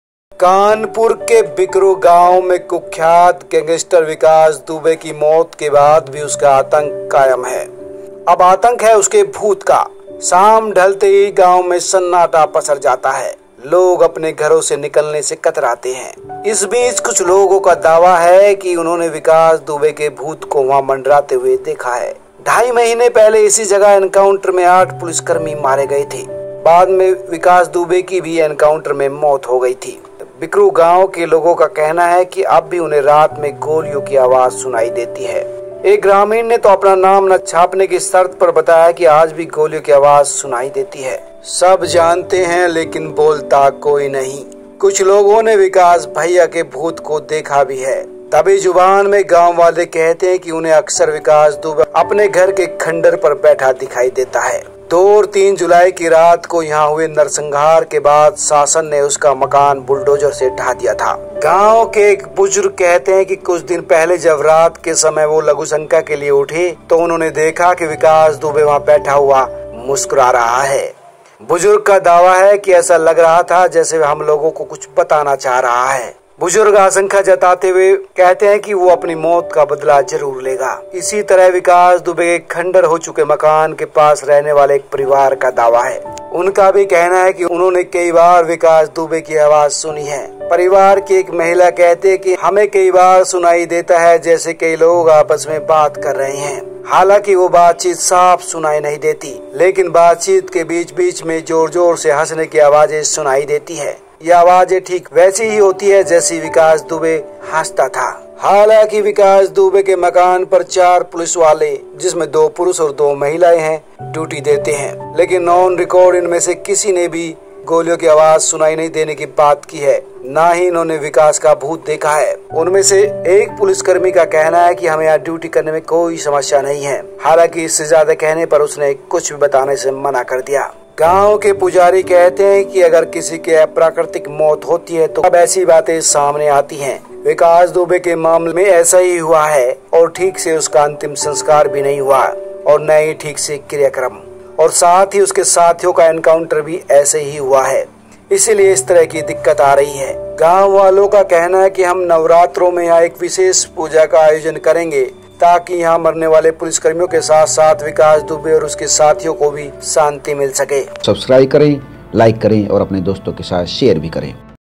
Vikas Dubey,घर के खंडहर पर बैठा रहता है दुबे का भूत! गोलियों की आवाजें,हंसी के ठहाके देते हैं सुनाई
बिकरू में शाम होते ही विकास दुबे के 'भूत' से डर कर घरों में छुप जाते हैं गांववाले बोले- गोलियों की आवाजें,हंसी के ठहाके देते हैं सुनाई, दिखता है विकास दुबेकई गांववालों ने दावा किया है कि विकास दुबे खंडहर पर बैठा रहता है। वह कुछ बताना चाहता है। कुछ लोगों ने यह तो भी कहा है कि उन्‍हें रात में गोलियों की आवाजें सुनाई देती हैं।
Many villagers have claimed that Vikas Dubey sits on the ruins. He wants to tell something. Some people have even said that they hear gunshots at night. His terror continues even after the death of notorious criminal Vikas Dubey in Bikeru village of Kanpur. Now there is terror of its ghost, as the evening sets in, there is a silence in this village, people hesitate to get out of their homes. Meanwhile, some people have claimed that they have seen the ghost of Vikas Dubey there. Two and a half months ago, 8 policemen were killed in the encounter at the same place, later Vikas Dubey also died in the encounter. People of Bikeru say that they still hear gunshots at night. One villager did not mention his name but said, 'Even today the sound of gunfire is heard. Everyone knows but no one speaks Some people have even seen the ghost of Vikas Bhaiya. The villagers say that they often see Vikas Dubey sitting on the ruins of his house. After the massacre here on the night of 2 and 3 July, the government demolished his house with a bulldozer.